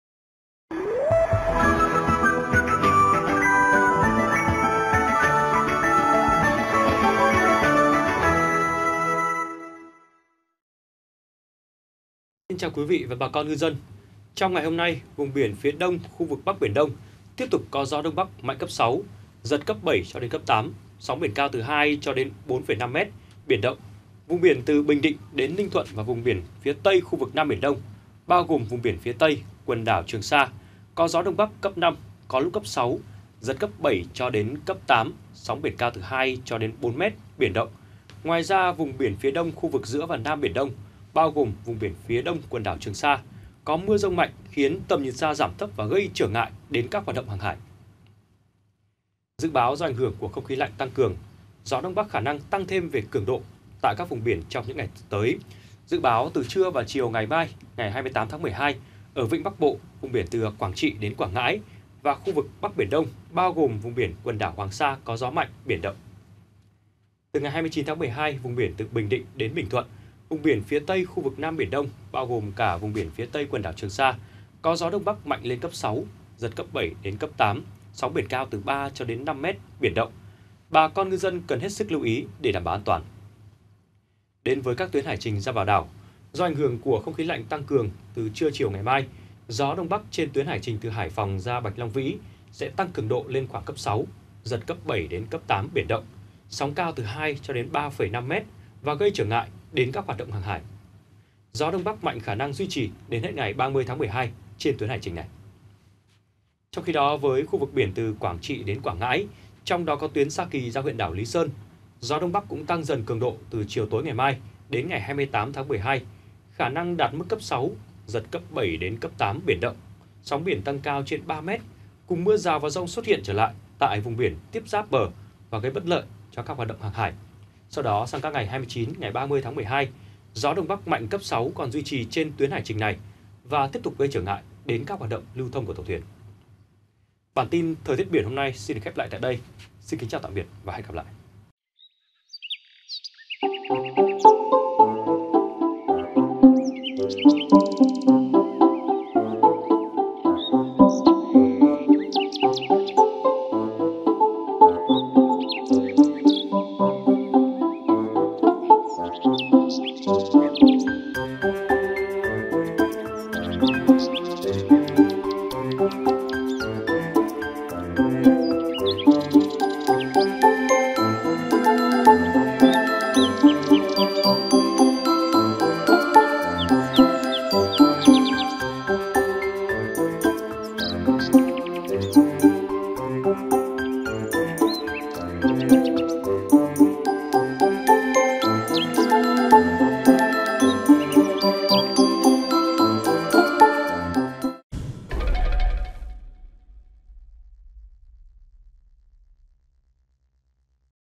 Thời tiết biển 27/12/2022: Gió Đông Bắc cấp 6, giật cấp 7-8| VTC14
Dự báo do ảnh hưởng của không khí lạnh tăng cường,  gió đông bắc khả năng tăng thêm về cường độ tại các vùng biển trong những ngày sắp tới, dự báo từ trưa và chiều ngày mai, ngày 28/12, ở vịnh Bắc Bộ, vùng biển từ Quảng Trị đến Quảng Ngãi và khu vực Bắc Biển Đông (bao gồm vùng biển quần đảo Hoàng Sa) có gió mạnh, biển động.